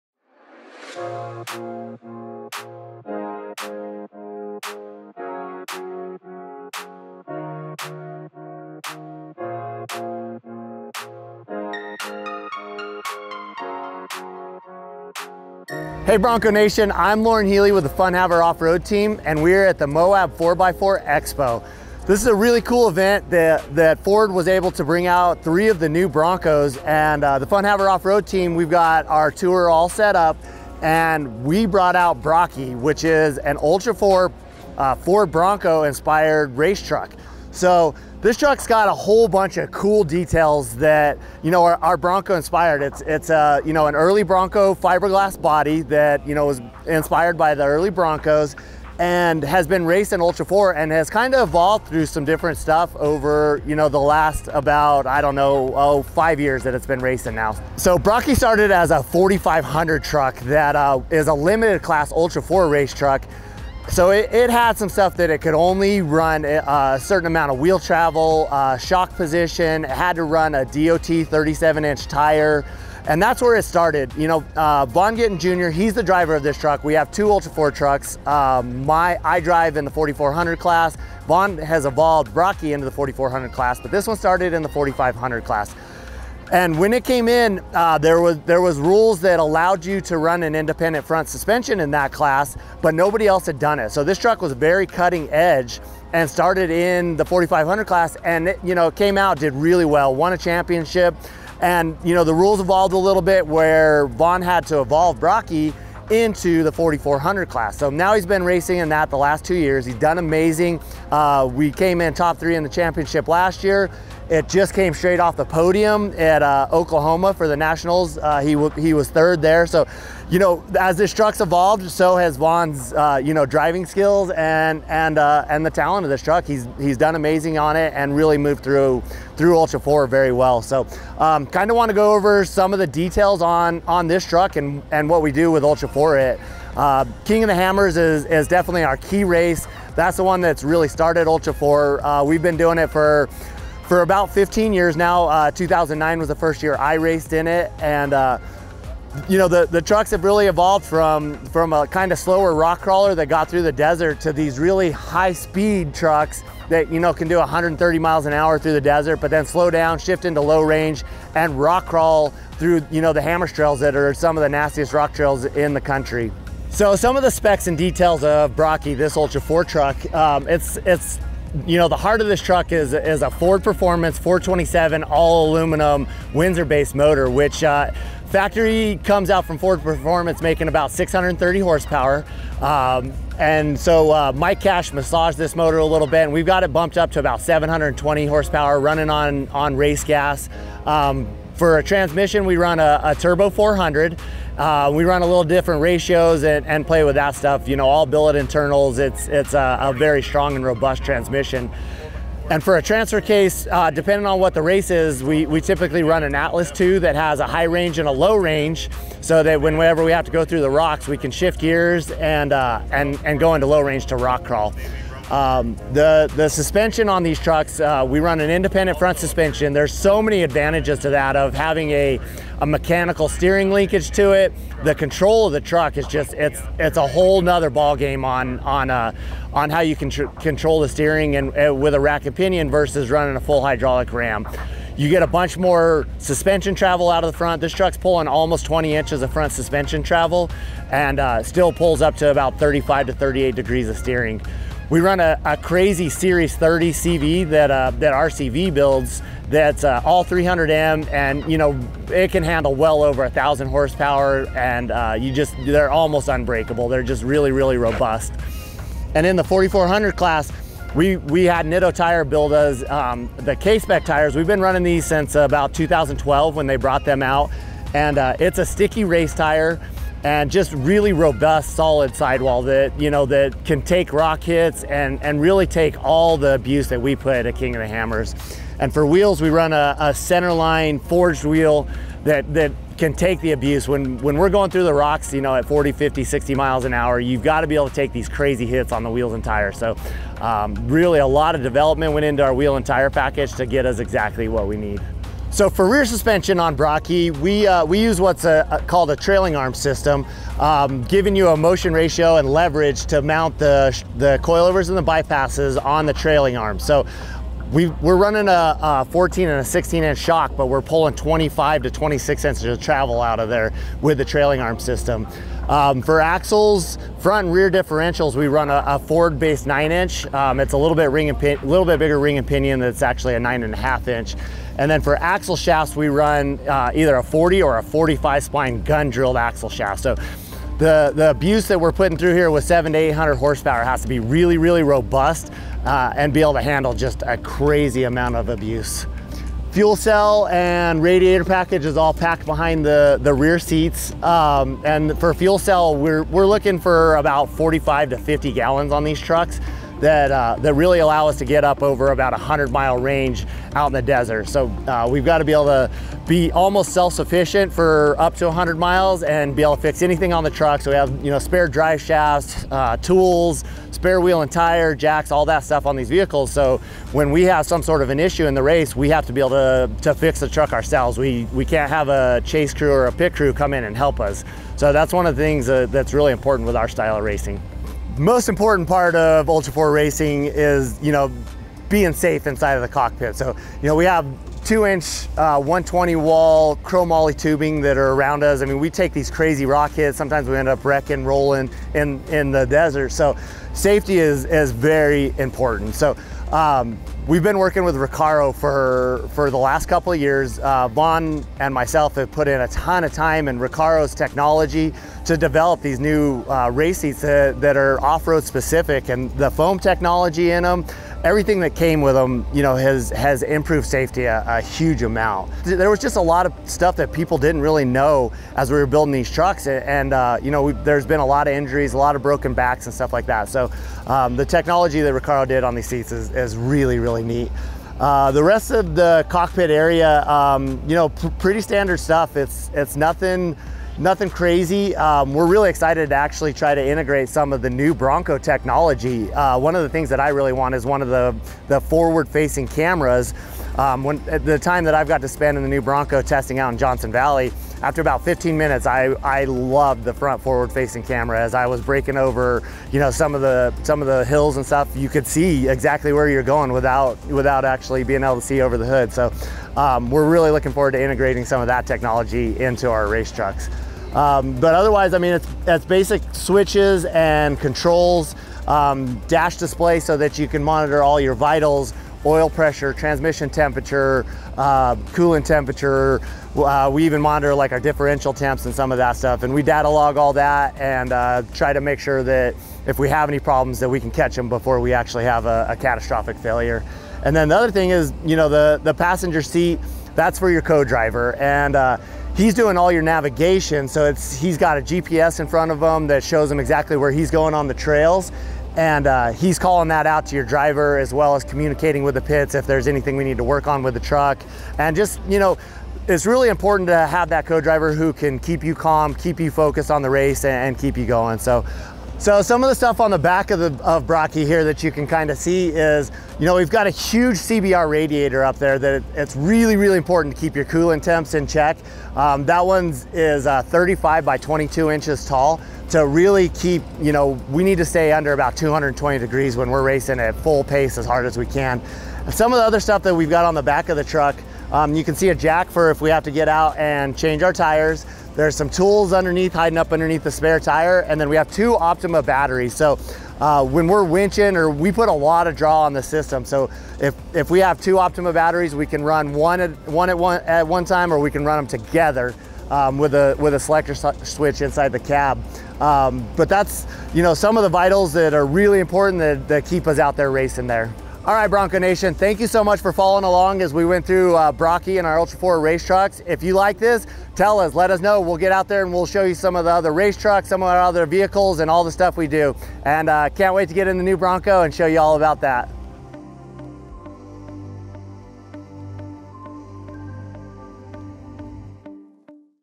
Fun-Haver Off Road's Brocky Walk-Around with Loren Healy | Bronco Nation
Bronco Nation met up with Loren Healy of the Fun-Haver Off Road team to get a walk-around of Brocky, the Ultra4 race truck inspired by the Ford Bronco. Watch as he goes over the highlights, tech specs, and details that make this race truck so impressive.